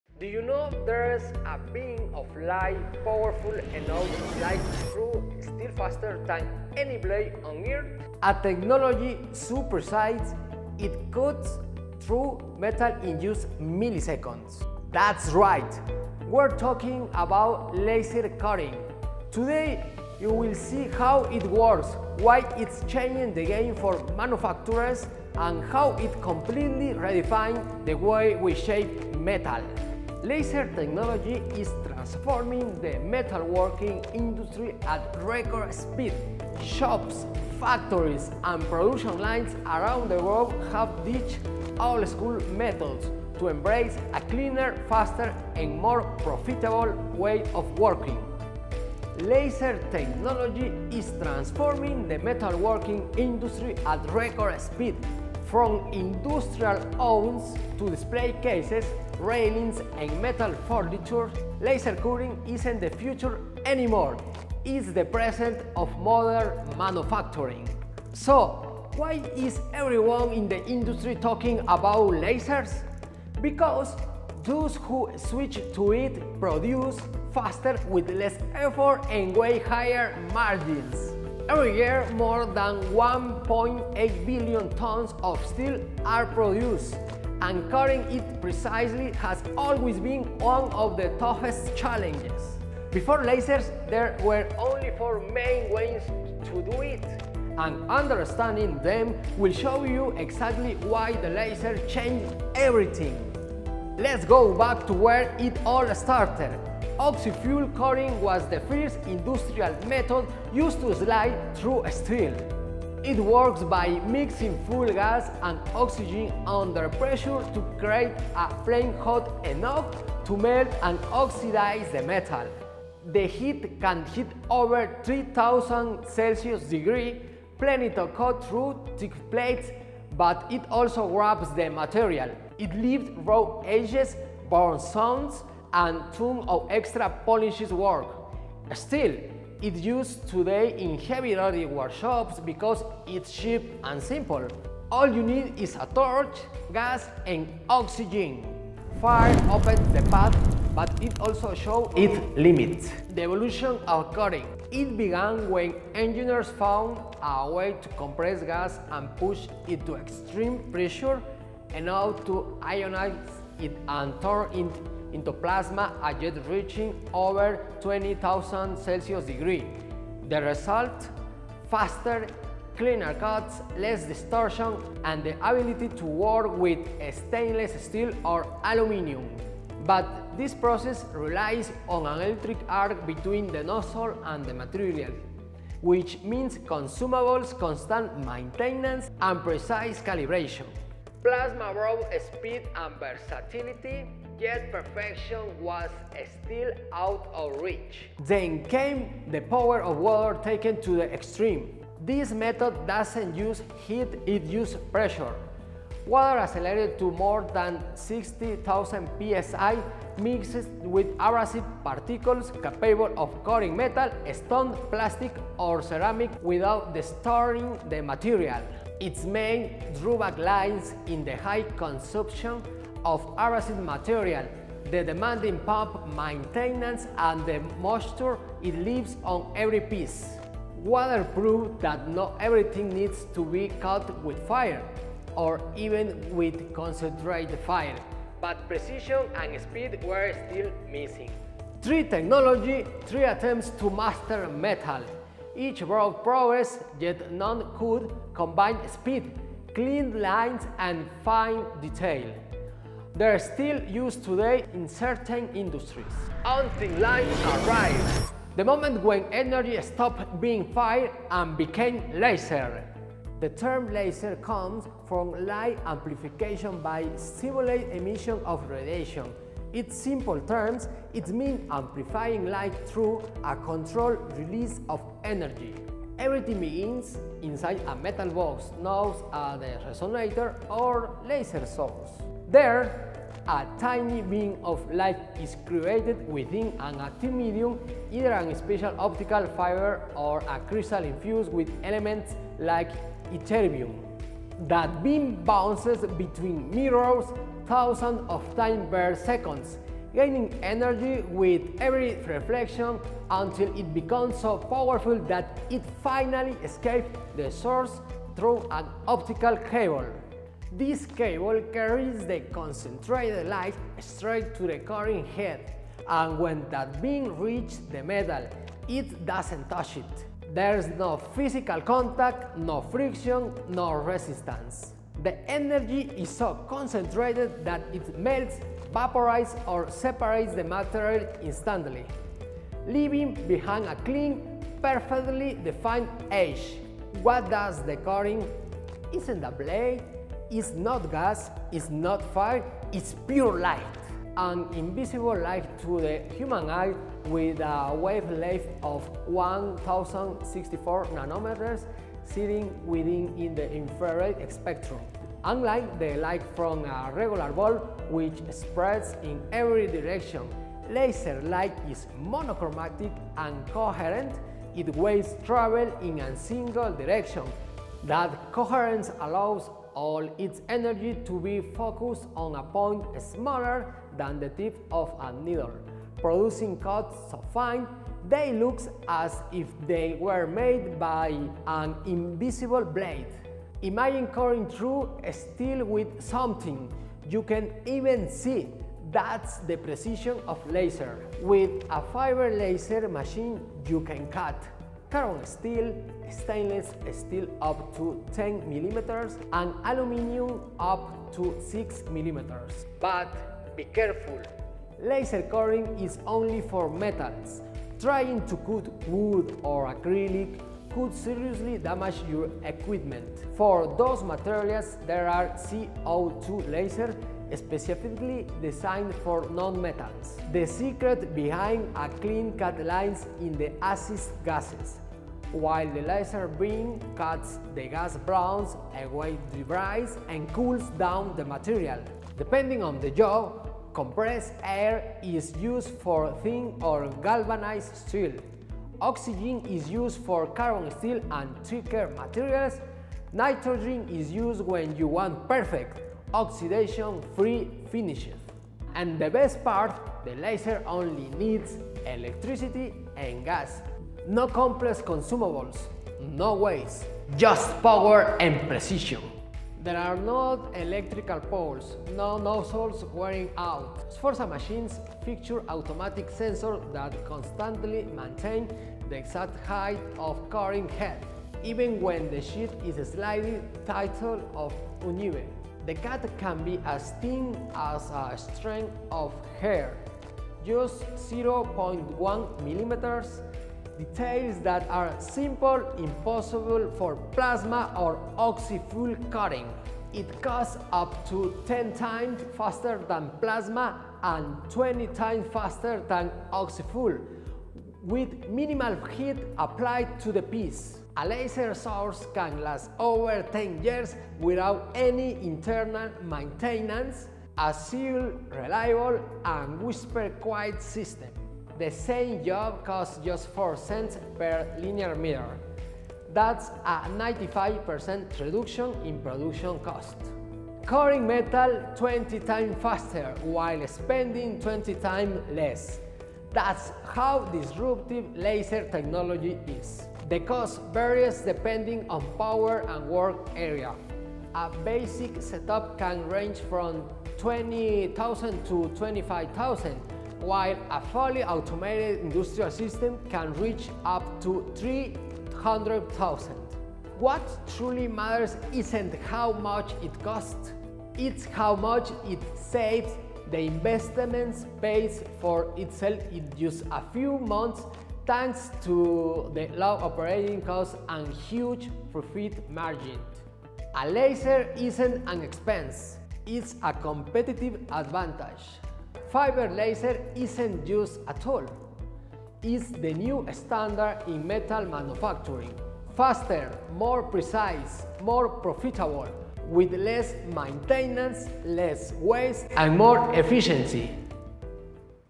Fiber Laser Cutting Explained: Why It’s Replacing Plasma & Waterjet
What’s the best technology for cutting metal? In this video, we compare four leading methods: laser cutting, plasma cutting, oxy-fuel, and waterjet. Discover the pros and cons of each process in terms of precision, speed, cost, material waste, and safety.

Whether you’re a manufacturer, metalworker, or industrial engineer, understanding these technologies is key to choosing the right solution.

🚀 See why more companies are switching to FORZA Laser for next-level performance and ROI.